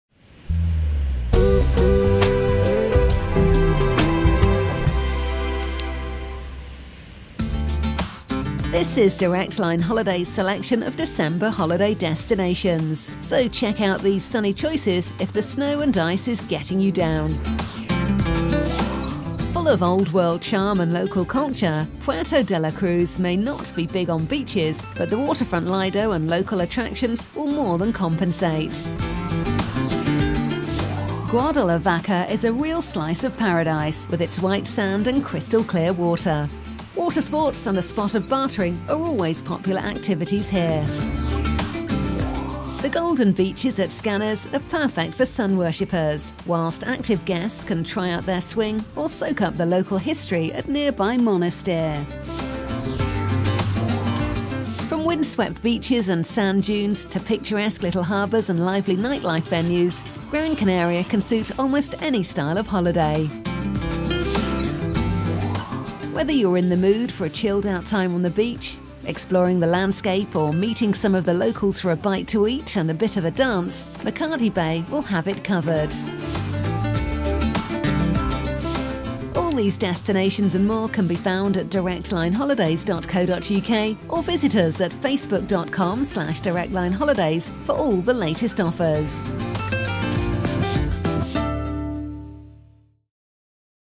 December Holiday Destinations
Watch Directline Holidays video of our selection of top destinations for a holiday in December! Get some ideas on destinations to top up your tan while the UK shivers in winter.